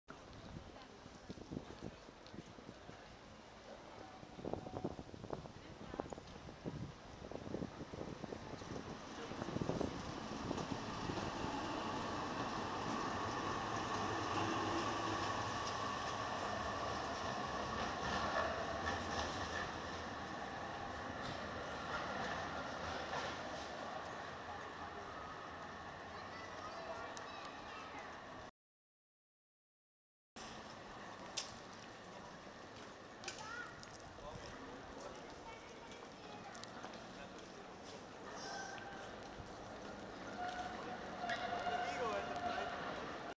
Letzter Betriebstag Gleis MTS-Nord
Tw 31 der IVB am letzten Betriebstag des Gleises durch die Fußgängerzone in der nördlichen Maria-Theresien-Straße in Innsbruck.